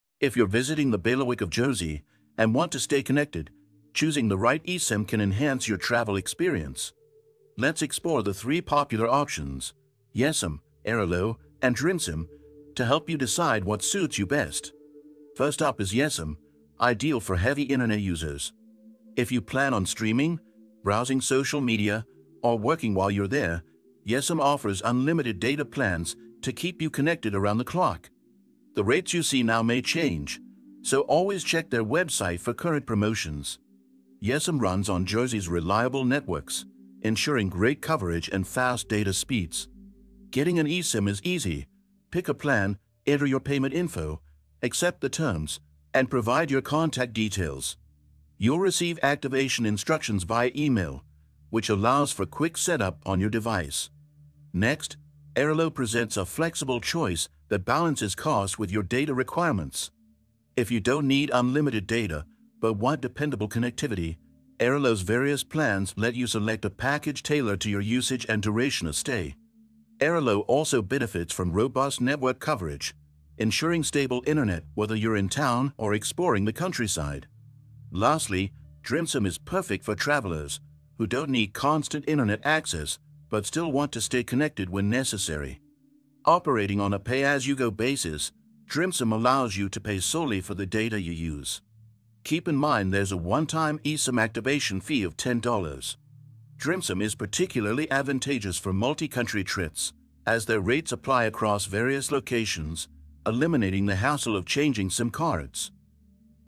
Best eSIM options for Bailiwick of Jersey, the best plans for travelers
Heading to Bailiwick of Jersey and unsure which eSIM to choose? In this video, we’ll explore the three best eSIM options: Yesim, Airalo, and Drimsim. These providers offer different plans depending on how you use the internet while traveling. Whether you need unlimited data or just occasional access, we’ve got you covered.

Let’s get into the details and find out which plan works best for your trip! Unlimited, limited and pay-as-you-go data  eSIM plans.